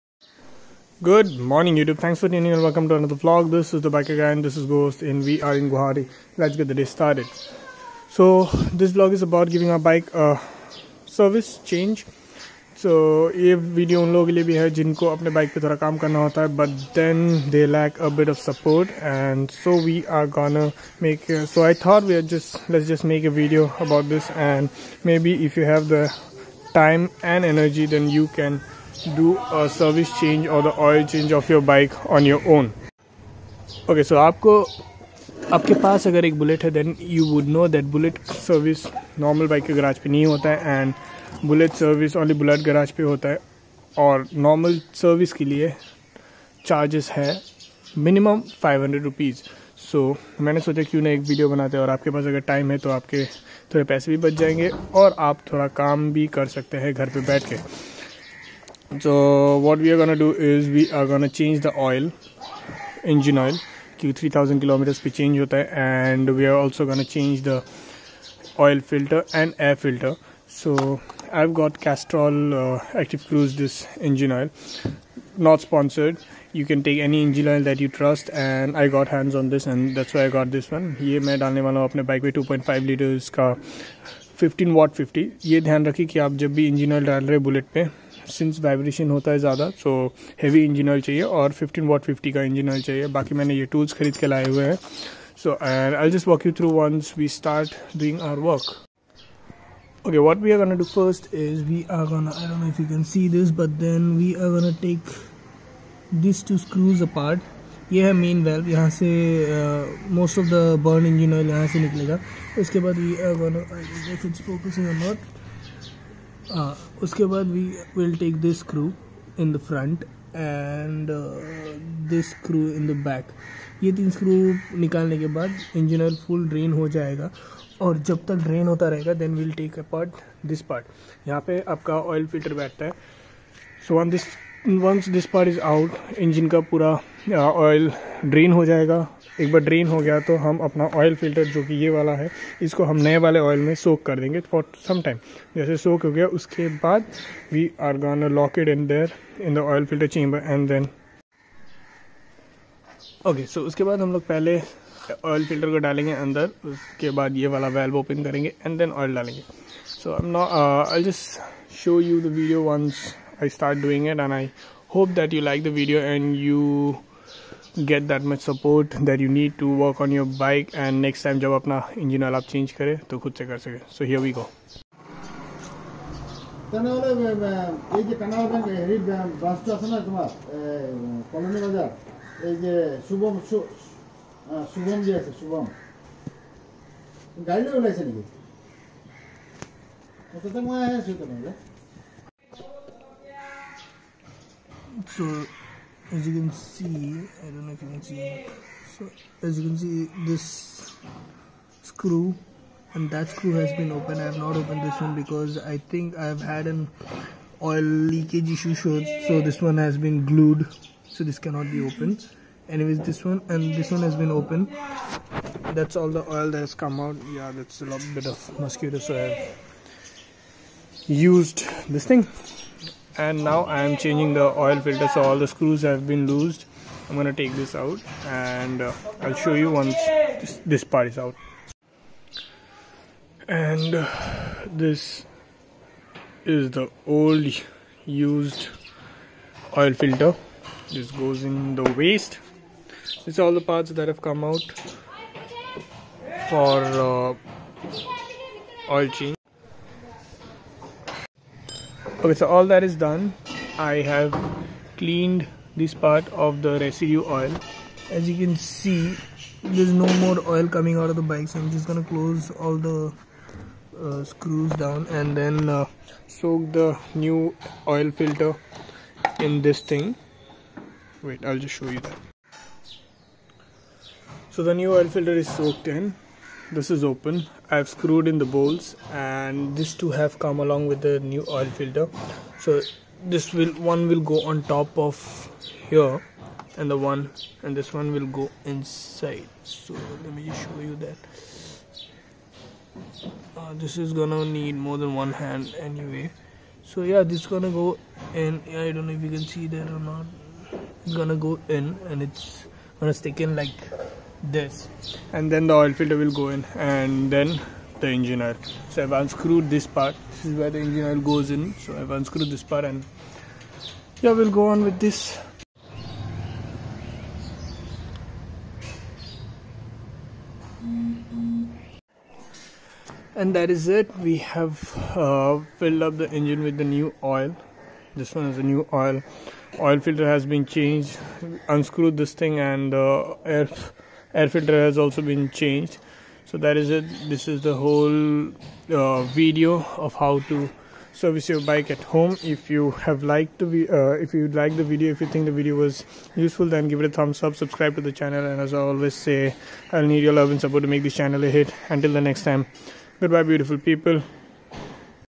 How to change bullet 350 engine oil | Bike Service
If you want to start working on your bike then this will be a great start. check out my video to know how to service your bike all by yourself